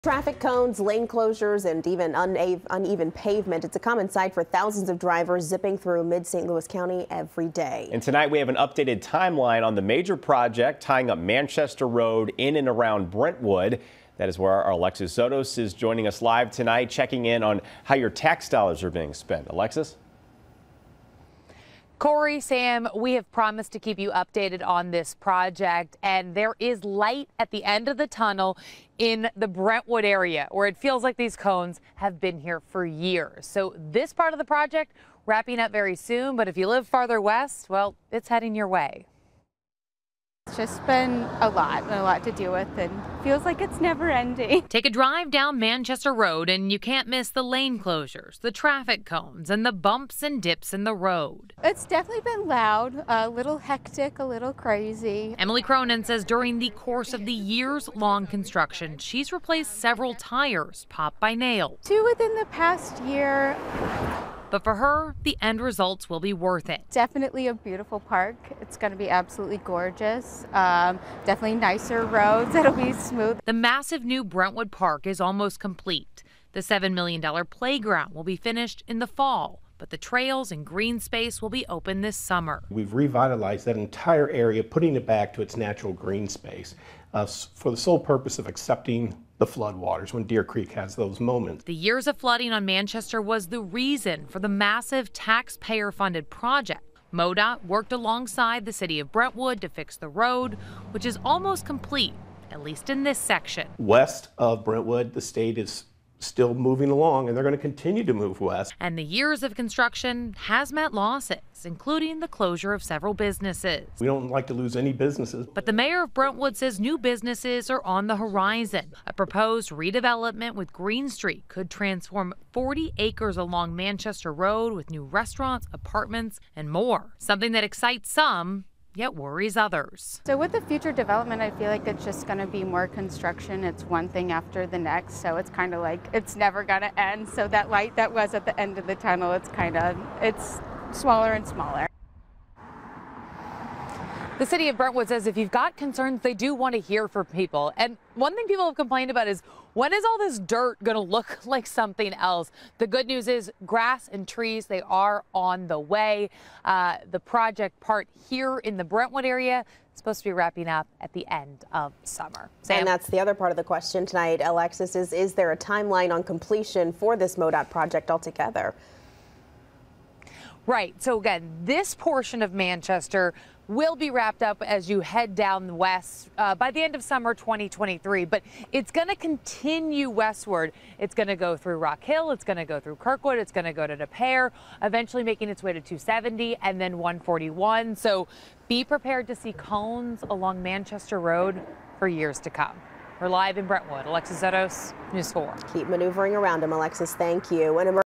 Construction on Manchester Road wrapping this summer in Brentwood but will continue through West...
For people who live, work and drive through Brentwood, there is a light at the end of the tunnel when it comes to construction.